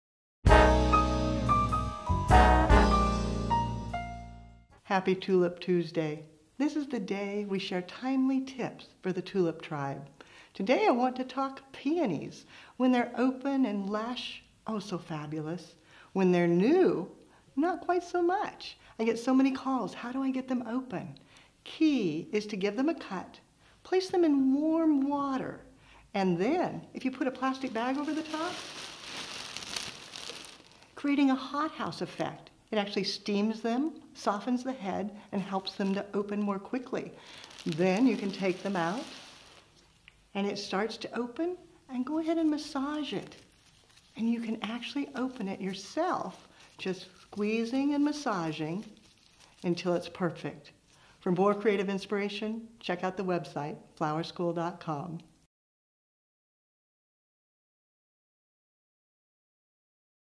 Opening Peony Blossoms
A fully open peony is one of the most beautiful flowers in the world.  But, sometimes the peonies that you need for a design are not yet open.  In this Tulip Tuesday how-to video Leanne demonstrates the techniques for speeding the process of opening peony blossoms.  Enjoy!